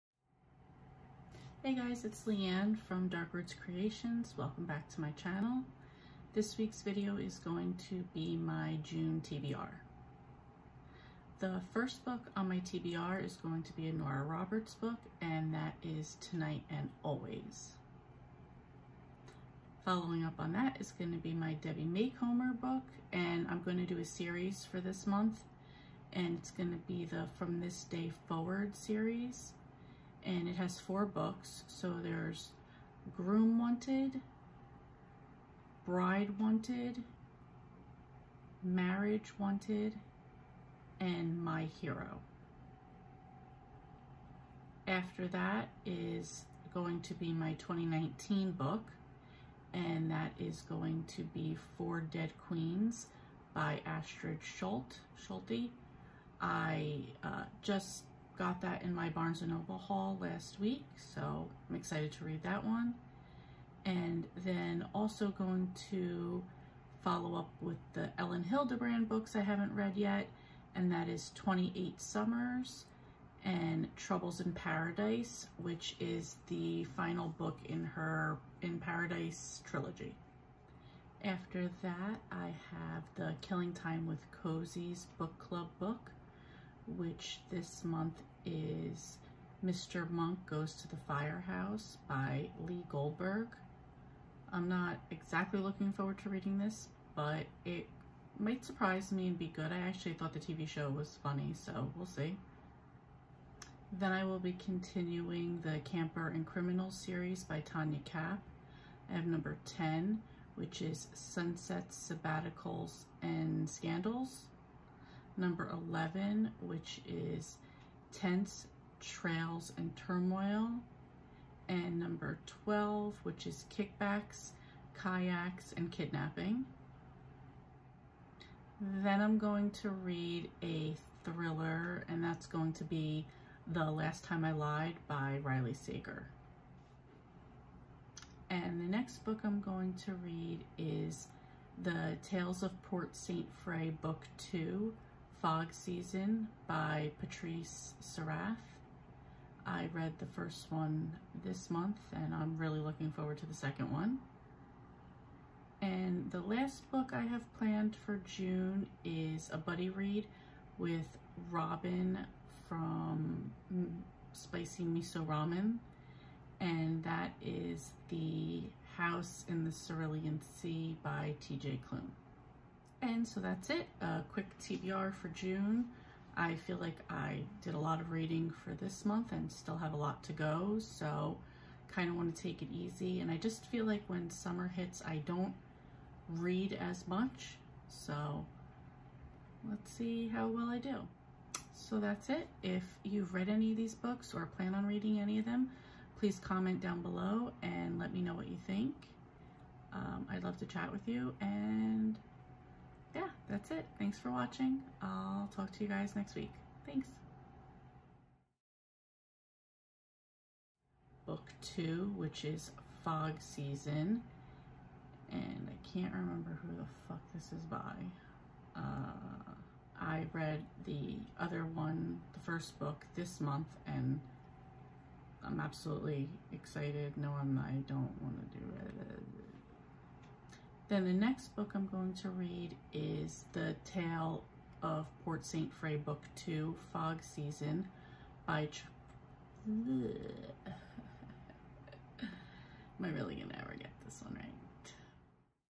My June TBR
Books mentioned in this video:
Tonight & Always by Nora Roberts
Groom Wanted by Debbie Macomber
Bride Wanted by Debbie Macomber
Marriage Wanted by Debbie Macomber
My Heroby Debbie Macomber
Four Dead Queens by Astrid Scholte
28 Summers by Elin Hilderbrand
Troubles in Paradise by Elin Hilderbrand
Mr. Monk Goes to the Firehouse by Lee Goldberg
Sunsets, Sabbatical & Scandal by Tona Kappes
Tents, Trails & Turmoil by Tonya Kappes
Kickbacks, Kayaks & Kidnapping by Tonya Kappes
The Last Time I Lied by Riley Sager
Fog Season by Patrice Sarath
House In the Cerulean Sea by TJ Klun

Channels mentioned in the video:
Robin @ SpicyMisoRamen

Killing Time with Cozies Book Club